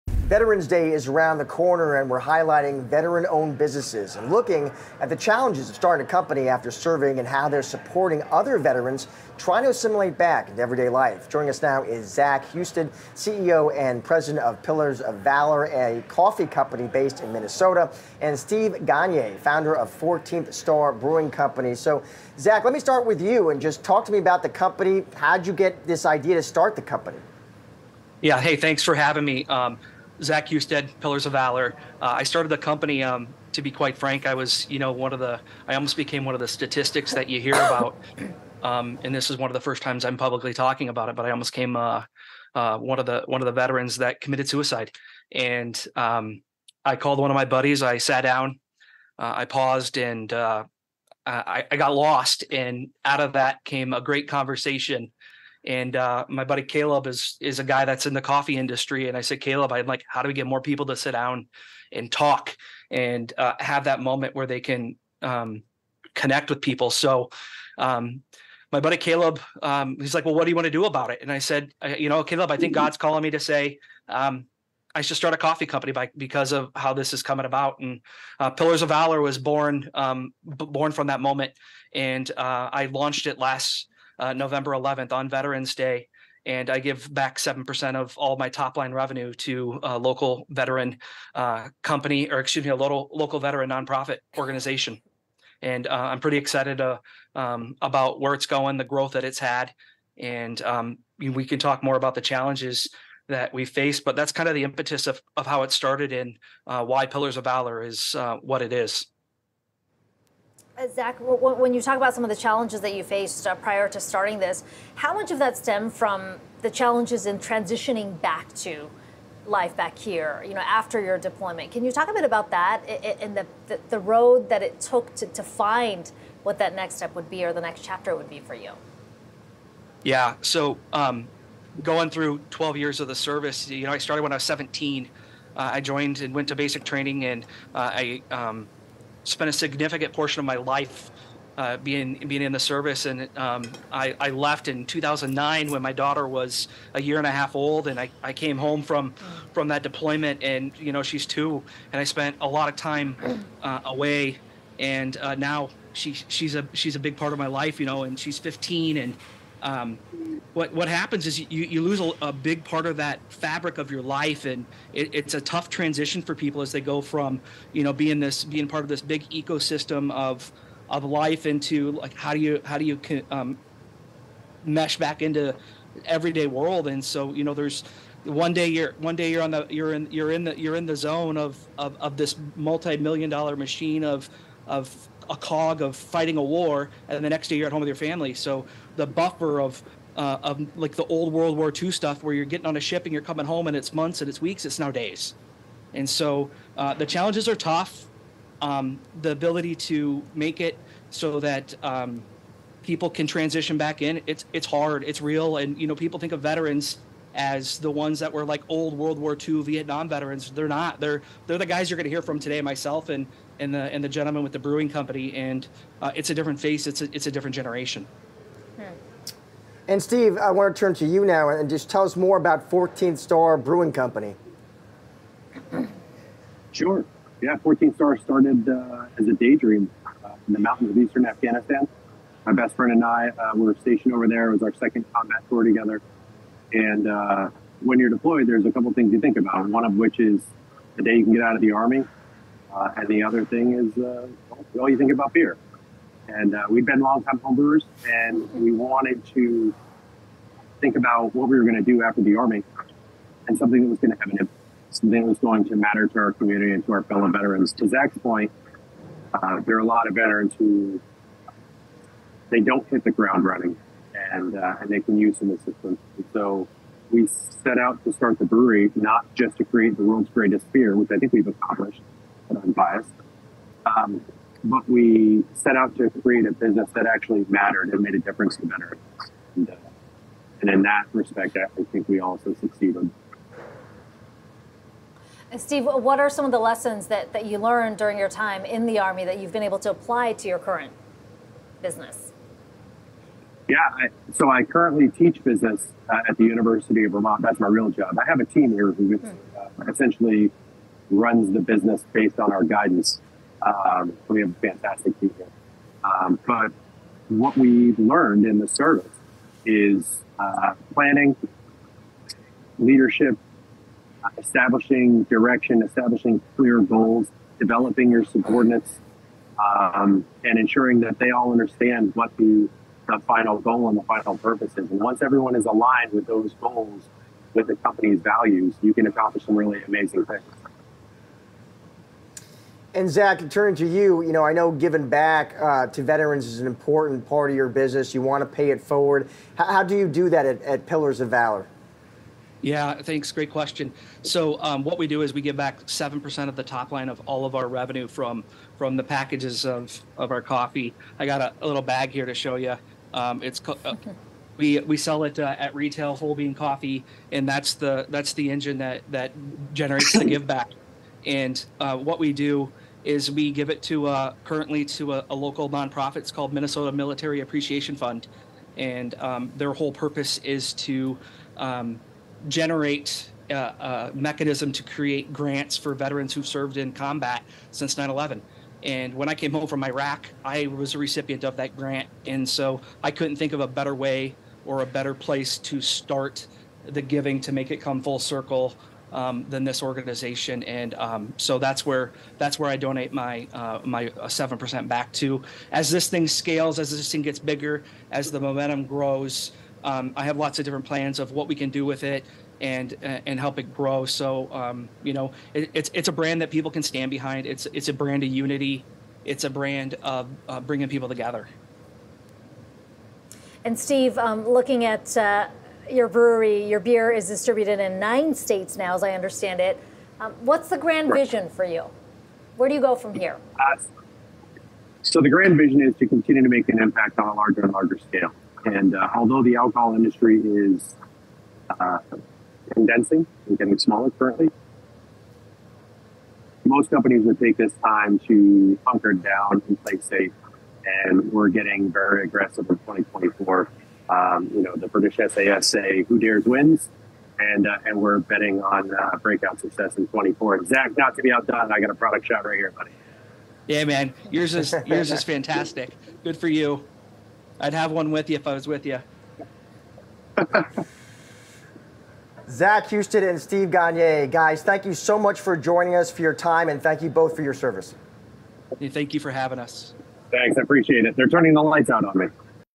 Veterans discuss launching small businesses after leaving military and overcoming challenges
With Veterans Day approaching on November 11, 2023, Yahoo Finance Live spoke with two veteran entrepreneurs about the challenges of starting a business post-service and supporting fellow veterans.
Zach Husted, CEO of Pillars of Valor, launched his coffee company after struggling with PTSD that nearly led to suicide. A conversation with a close friend sparked the idea to give veterans a space to openly discuss their experiences. Pillars of Valor, launched on Veterans Day 2022, donates 7% of revenue to the Minnesota Military Appreciation Fund to aid veterans' transition to civilian life. Husted sees it as "full circle" to give back, having once received a grant from the fund himself.
Steve Gagner, CEO of 14th Star Brewing Co., recalled how his brewery began as a "daydream in the mountains of Afghanistan" about life after the army. He and a friend envisioned starting a brewery focused on the two things soldiers think about - leaving the military and beer. Now distributed in 9 states, 14th Star aims to continue making an impact supporting veterans and charities on a larger scale.
"What happens is you lose a big part of that fabric of your life and it's a tough transition for people as they go from, you know being part of this big ecosystem of life into like how do you mesh back into this world." Husted tells Yahoo Finance.